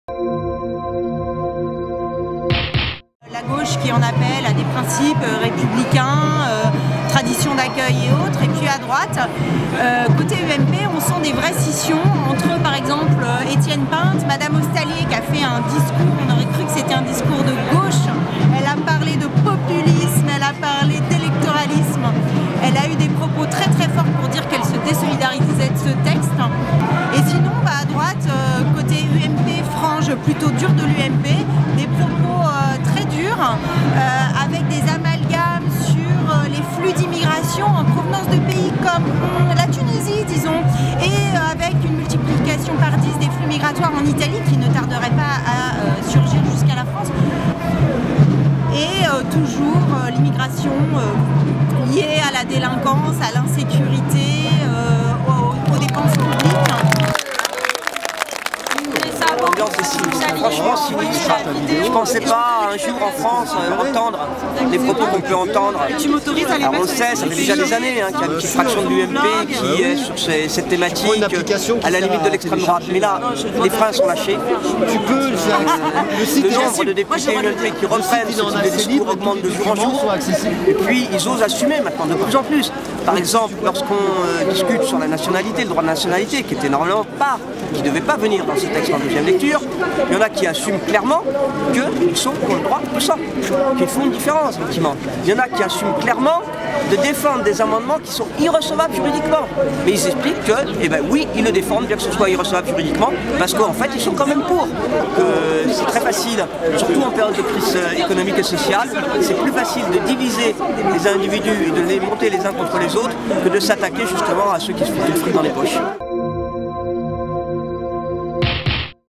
Immigration : le point sur le débat à l'Assemblée
Le projet de loi Besson sur l'Immigration est examiné ce jeudi en deuxième lecture à l'Assemblée nationale. Mercredi, les opposants au texte se sont réunis dans toute la France pour se faire entendre, espéraient encore obtenir des victoires ciblées. Reportage devant l'assemblée.